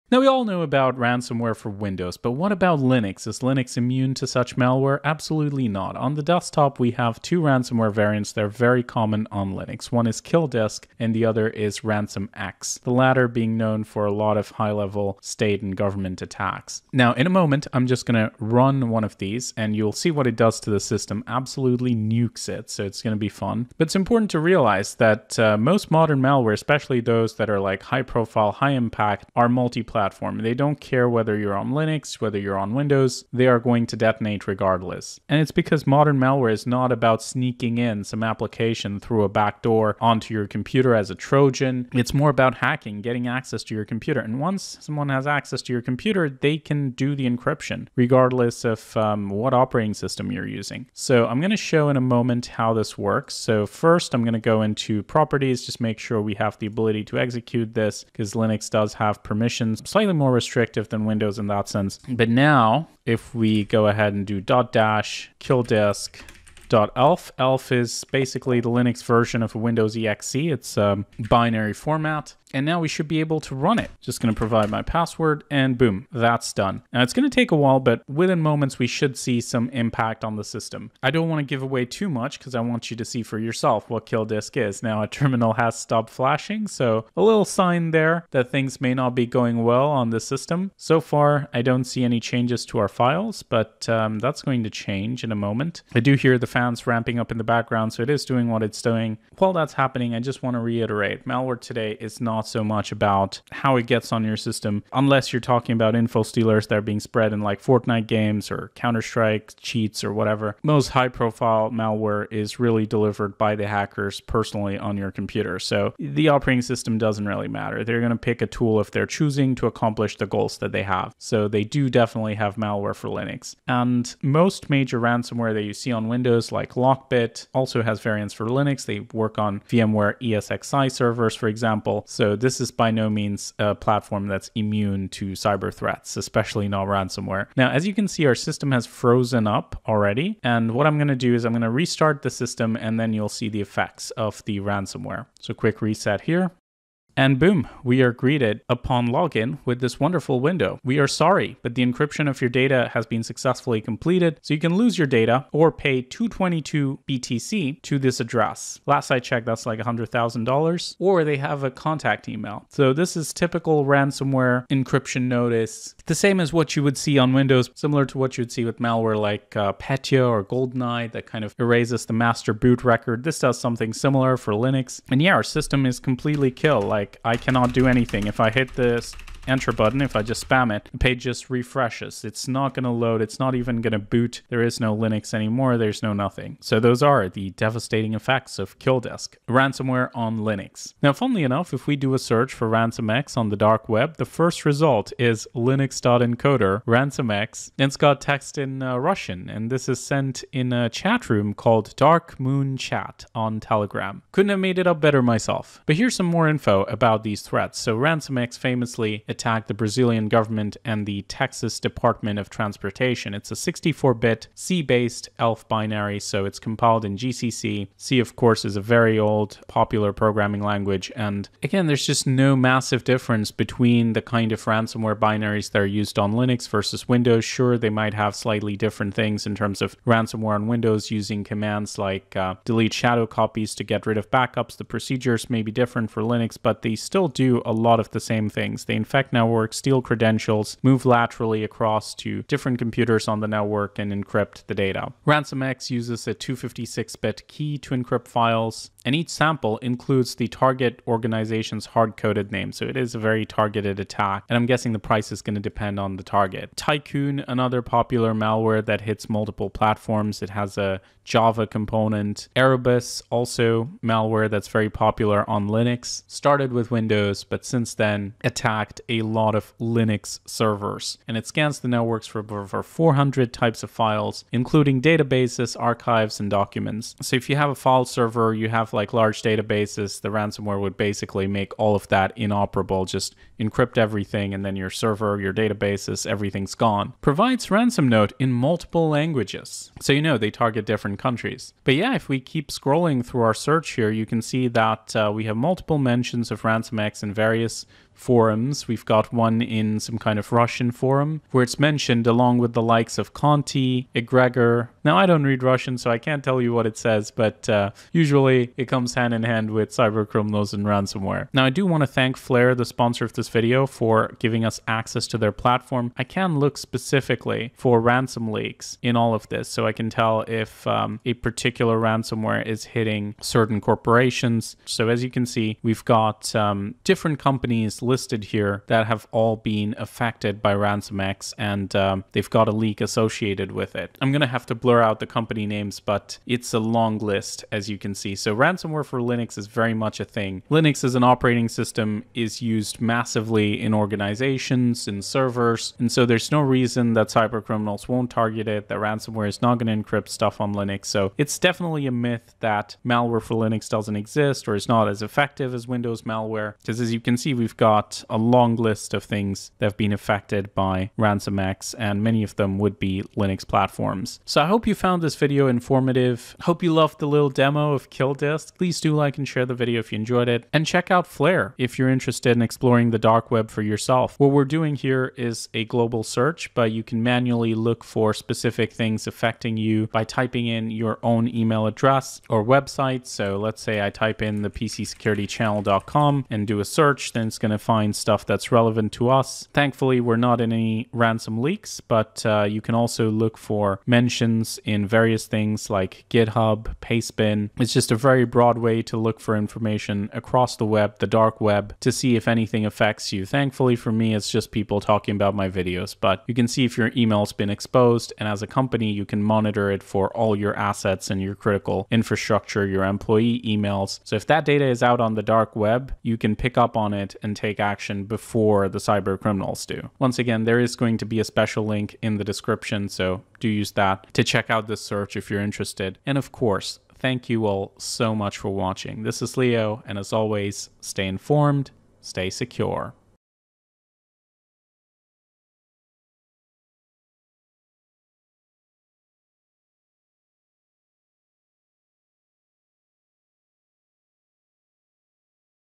Linux vs Windows: Ransomware
Linux Ransomware does exist and it may even be worse than Windows Ransomware in some cases. Linux Ransomware Killdisk, for instance destroys Linux as bad as any Windows virus.
Explore the dark web safely with a Flare trial and see if you have leaked data to cybercriminals (Sponsor)